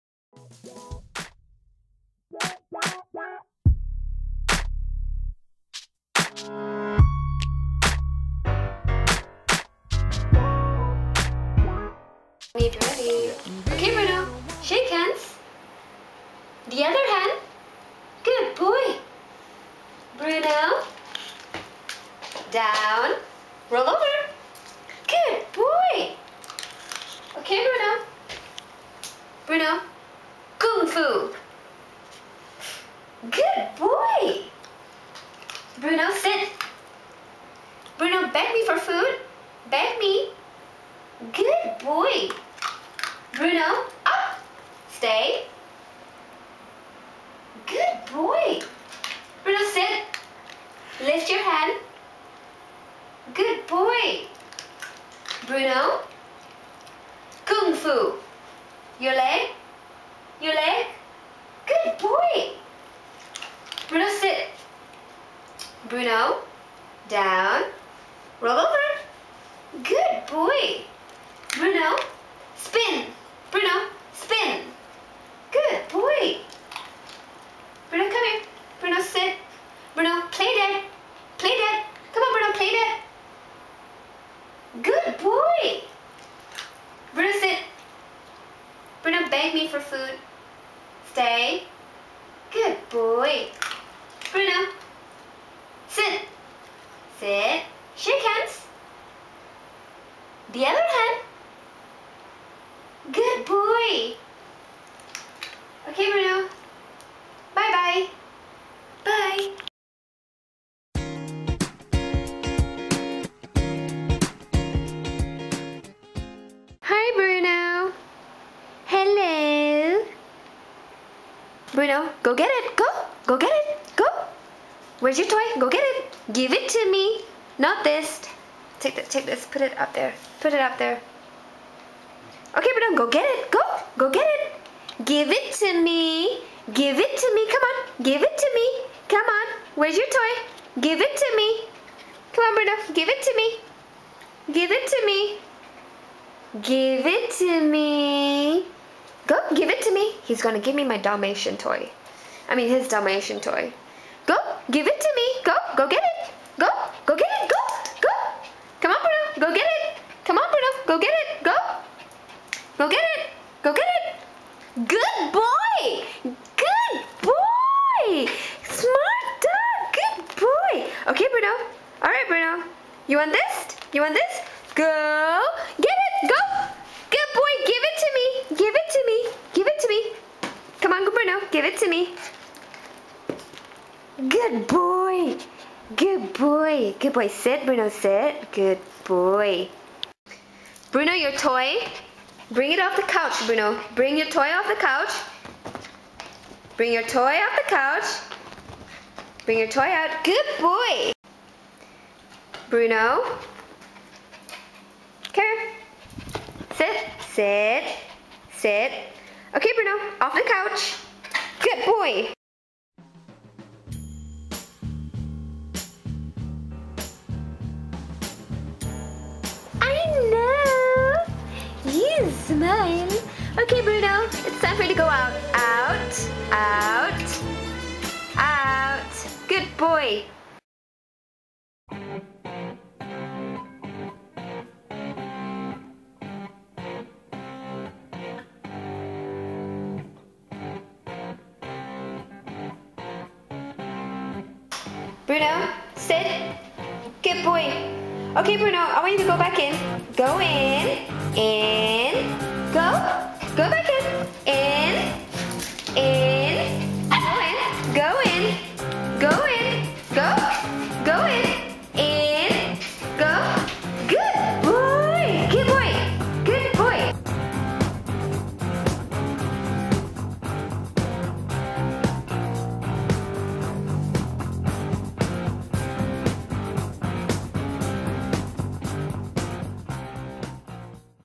Bruno!
Bruno, my 8 month old mini poodle doing tricks!